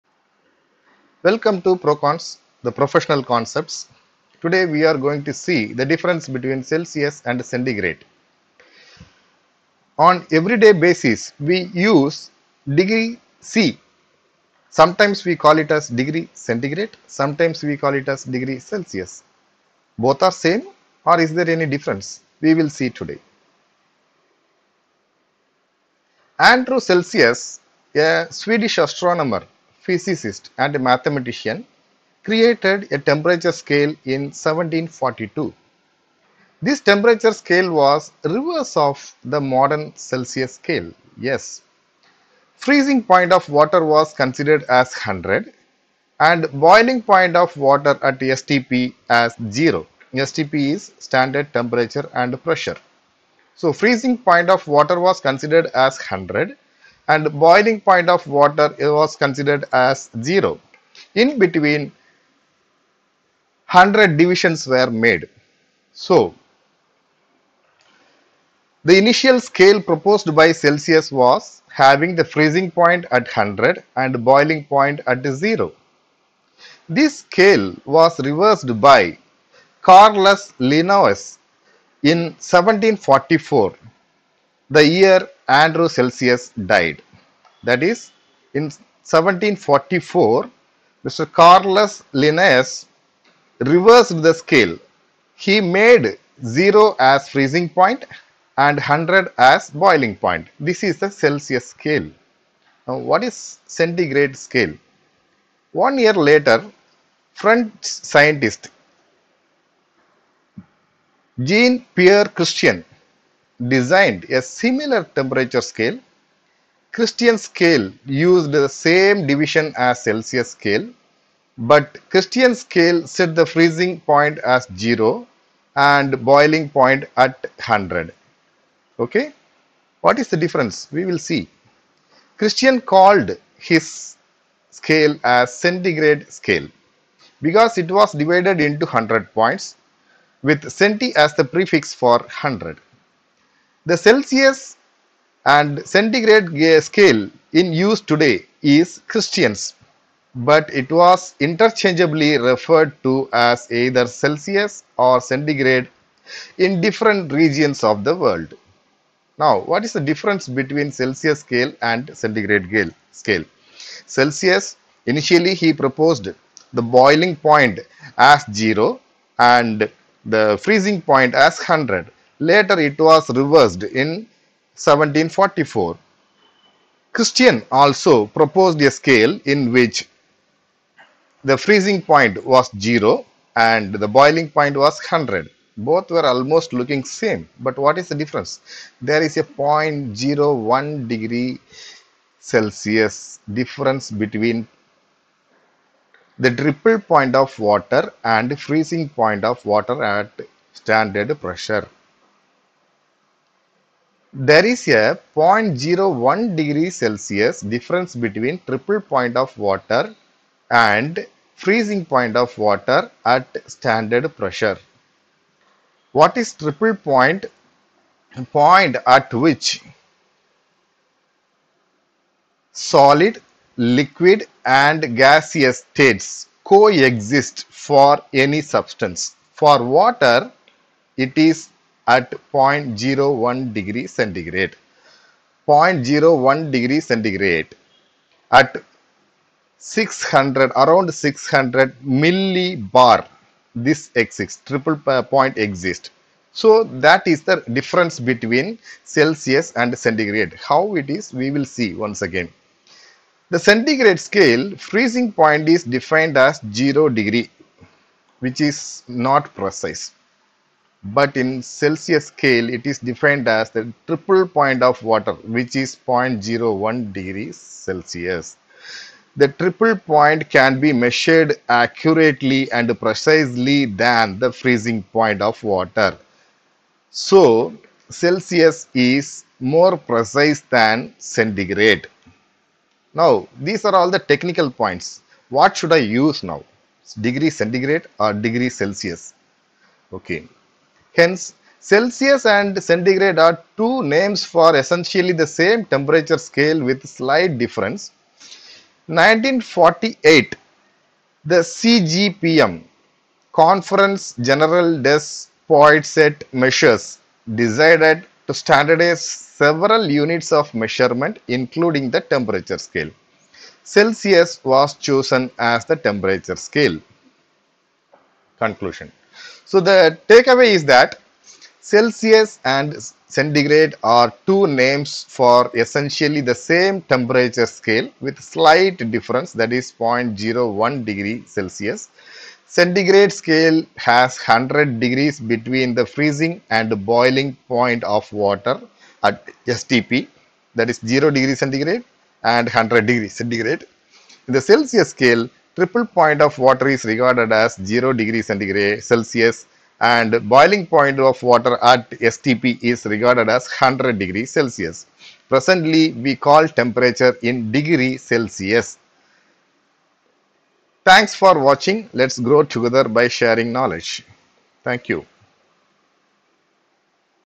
Thermal Basics | Celsius vs Centigrade scales
Difference between Celsius and Centigrade scales explained